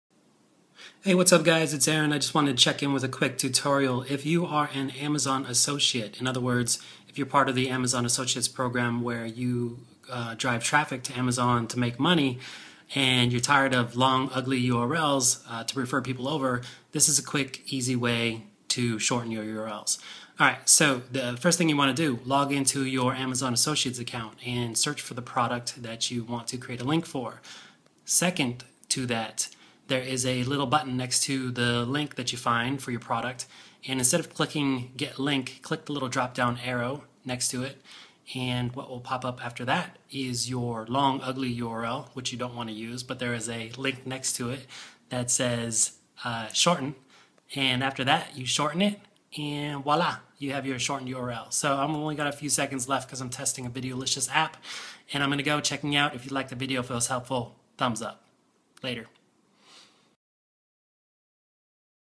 How to Shorten Amazon Affiliate Links without Bit.ly
Quick and direct way to shorten amazon affiliate links.

Even though this is a pretty easy, straight forward to shorten your affiliate links, it took me a while to figure out how to do it.

Why would you want to shorten your amazon affiliate links? They are much more user-friendly than the default links and probably more likely to get you clicks.

Sorry for the rapid fire video... testing a video app called Videolicious.

Screenshots were edited using the Skitch app.